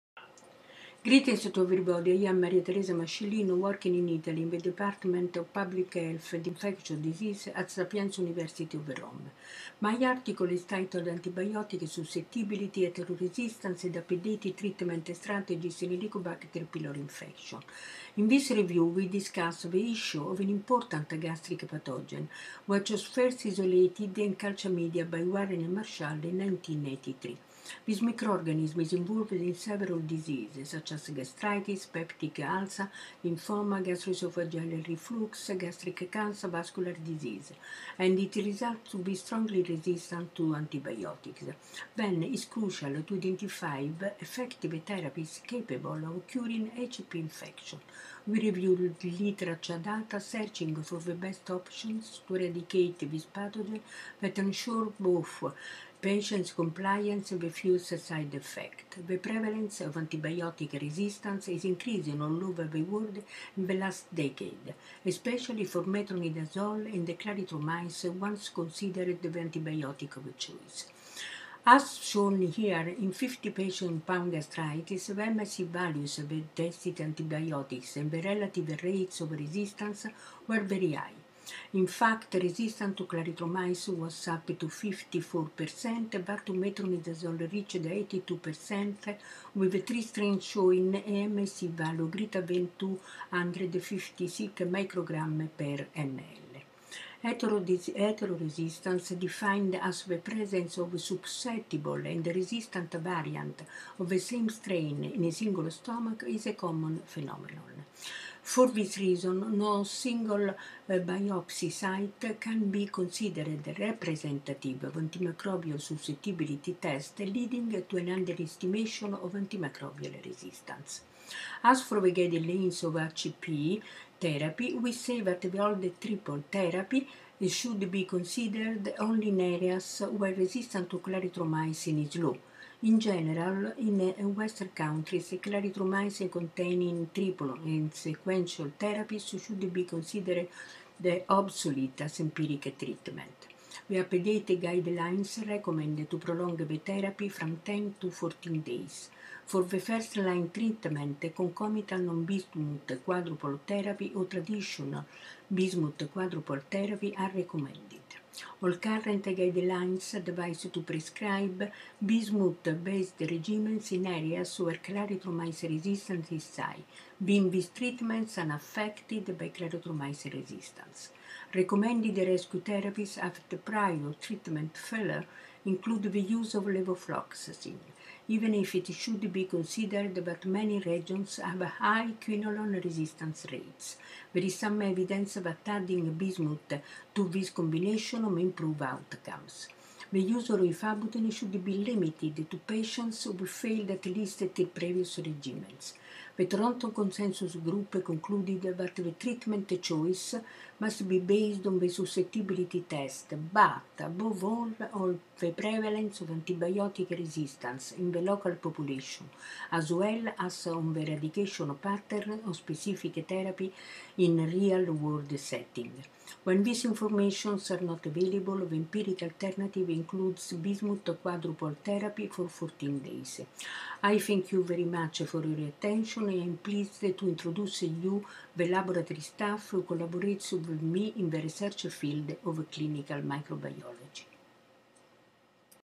Helicobacter pylori resistance and updated treatment strategies - Video Abstract 136240
Video abstract of review paper “Antibiotic susceptibility, heteroresistance, and updated treatment strategies in Helicobacter pylori infection”  published in the open access journal Drug Design, Development and Therapy by authors Mascellino, Porowska, De Angelis et al.

Abstract: In this review, we discuss the problem of antibiotic resistance, heteroresistance, the utility of cultures and antibiotic susceptibility tests in Helicobacter pylori (Hp) eradication, as well as the updated treatment strategies for this infection. The prevalence of antibiotic resistance is increasing all over the world, especially for metronidazole and clarithromycin, because of their heavy use in some geographical areas. Heteroresistance (simultaneous presence of both susceptible and resistant strains in different sites of a single stomach) is another important issue, as an isolate could be mistakenly considered susceptible if a single biopsy is used for antimicrobial tests. We also examined literature data regarding eradication success rates of culture-guided and empiric therapies. The empiric therapy and the one based on susceptibility testing, in Hp eradication, may depend on several factors such as concomitant diseases, the number of previous antibiotic treatments, differences in bacterial virulence in individuals with positive or negative cultures, together with local antibiotic resistance patterns in real-world settings. Updated treatment strategies in Hp infection presented in the guidelines of the Toronto Consensus Group (2016) are reported. These suggest to prolong eradication therapy up to 14 days, replacing the old triple therapy with a quadruple therapy based on proton pump inhibitor (PPI), bismuth, metronidazole, and tetracycline for most of the patients, or as an alternative quadruple therapy without bismuth, based on the use of PPI, amoxicillin, metronidazole, and clarithromycin. The new drug vonoprazan, a first-in-class potassium-competitive acid blocker recently approved in Japan, is also considered to be a promising solution for Hp eradication, even for clarithromycin-resistant strains. Furthermore, there is growing interest in finding new therapeutic strategies, such as the development of vaccines or the use of natural resources, including probiotics, plants, or nutraceuticals.